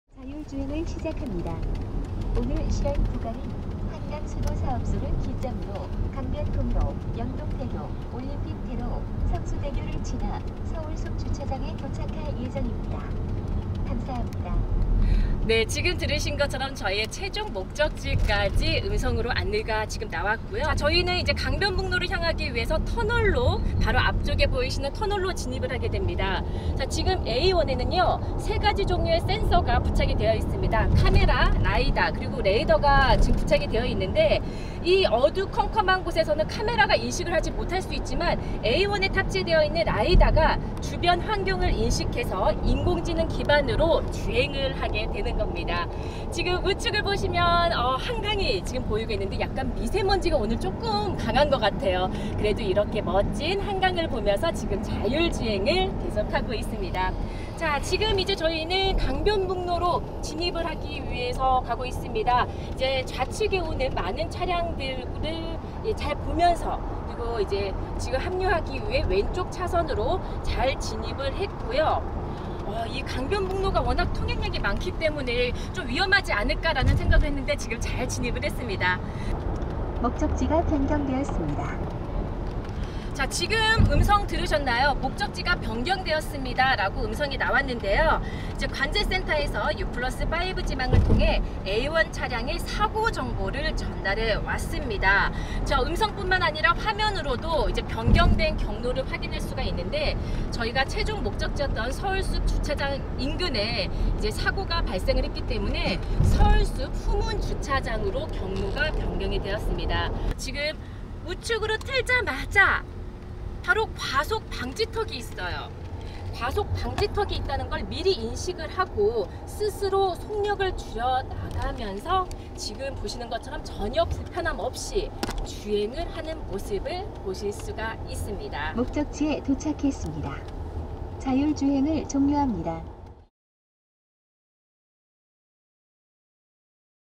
5G 자율주행 차량, 서울 도심서 '척척' '쌩쌩' 사람만큼 능숙하게 달렸다
LG유플러스는 이날 한양대 자동차전자제어연구실 ‘ACE Lab(에이스랩)’과 함께 세계 최초로 5G(5세대) 기반의 자율주행 기술을 공개 시연했다.

자율주행은 크게 0~5까지 여섯 단계로 나뉜다. 이날 시연은  4단계 ‘고도 자율주행’ 기술로 운전자 개입이 전혀 없이 도심 8㎞를 20여 분간 달렸다. 사람이 타지 않고도 움직이는 5단계 ‘완전자율주행’의 직전 단계다. 5G 네트워크를 바탕으로 4단계 기술이 성큼 앞당겨진 셈이다. 미국에서 자율주행 택시 영업을 시작한 구글의 웨이모가 4단계와 5단계 사이의 기술로 영업을 하고 있다. 국내 자율주행 기술은 해외에 견줄 정도가 된다. 현대차그룹은 2020년까지 4단계, 2030년까지 ‘완전자율주행차’ 상용화를 목표로 하고 있다. 특히 2021년에는 자율주행 친환경 로봇택시를 시범 운영하겠다고 선언했다. 선우 교수는 “국내 자율주행 기술은 전 세계에서 가장 까다로운 신호체계를 갖고 있다는 국내 신호등을 97~98%가량 정확하게 인식할 정도여서 제도적 여건 등을 제외하면 기술적으로는 1~2년 내 상용화도 가능하다”고 말했다.